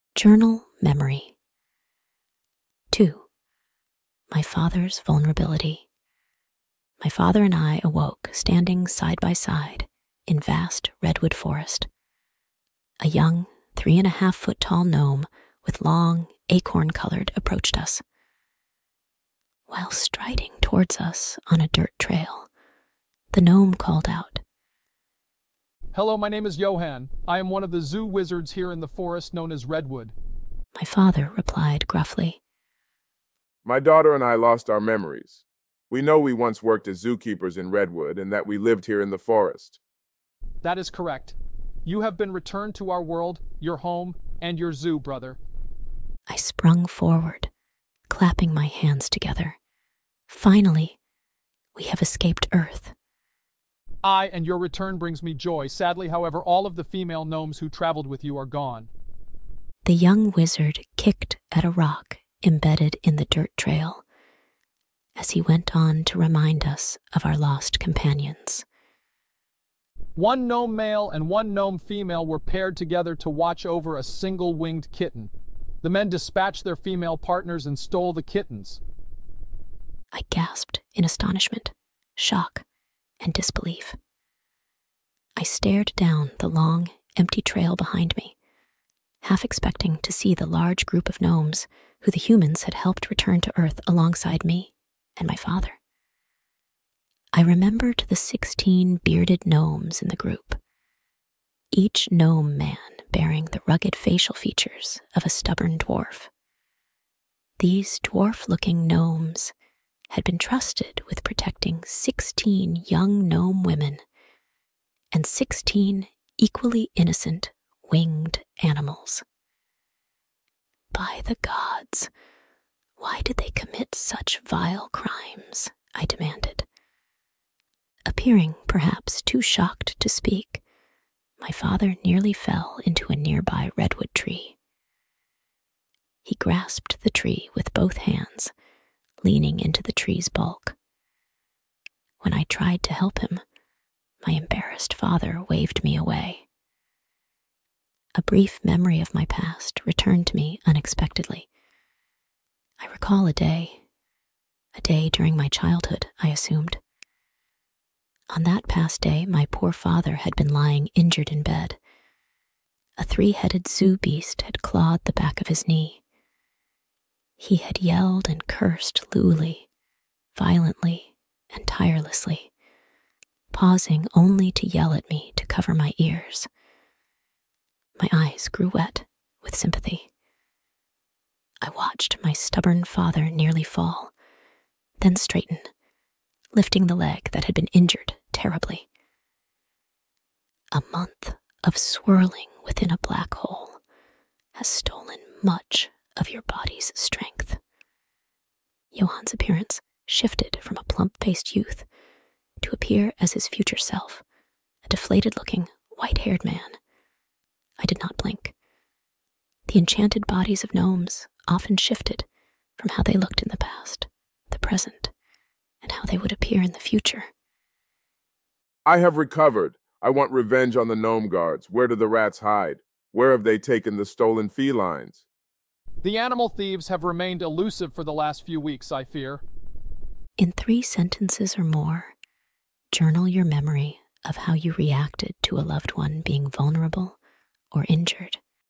Philosophical Question 2:Journal Response in Comments.
Philosophical Question 2 for kids and adults alike, given at the end of every short story chapter.
   Philosophical Question 2 at end of this chapter is: Celeste the teen gnome recalled a vulnerable time in her father's past.
     Do you believe that witnessing your loved ones, the main person who raised you the longest from your youngest age, birth or otherwise, being what most would consider physical or mentally unfit, not 100 percent themselves, due to uncontrollable circumstances ordue to self-inflicted pain, has or could alter your perception of that person?
    Example is below in the comments.